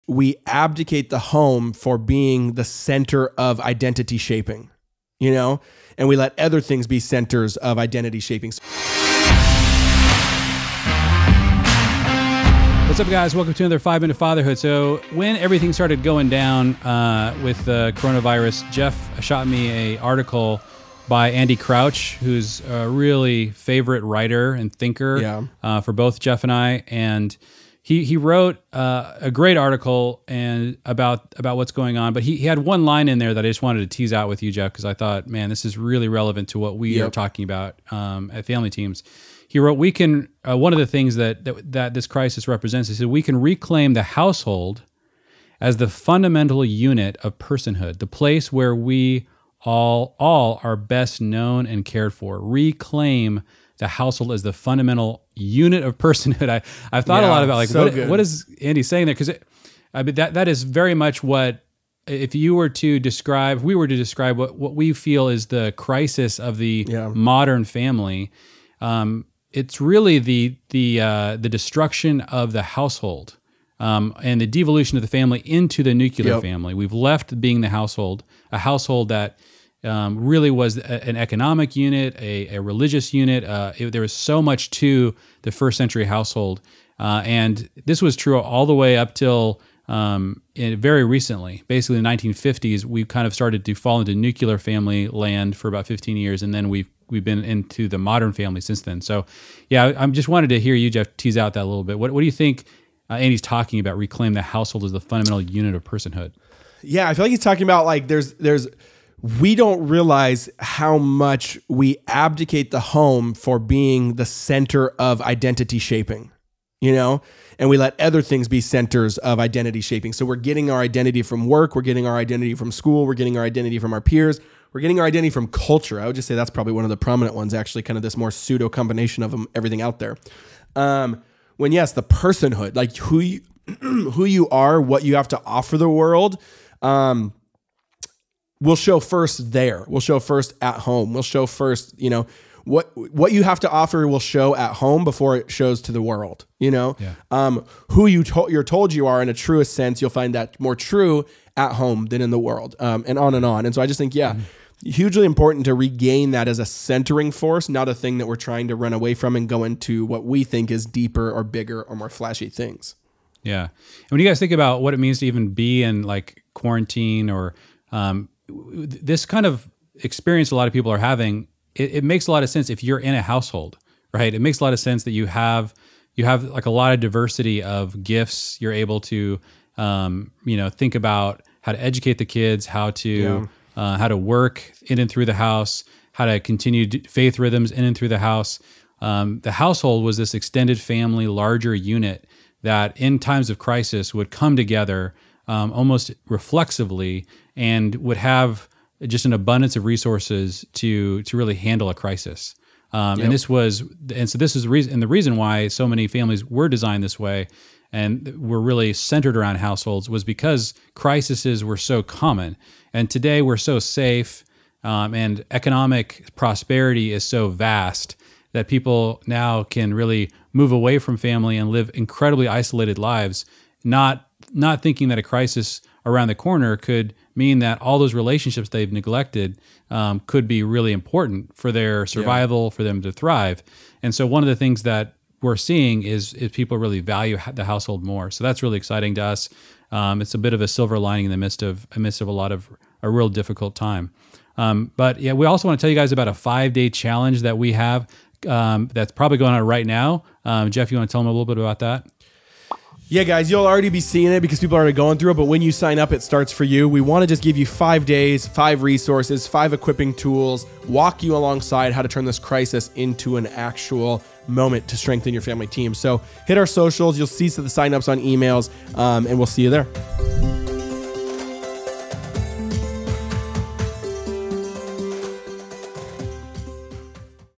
We Can Reclaim the Household Through This Crisis | Ep. 256
Jeff Bethke and Jeremy Pryor talk about reclaiming the household.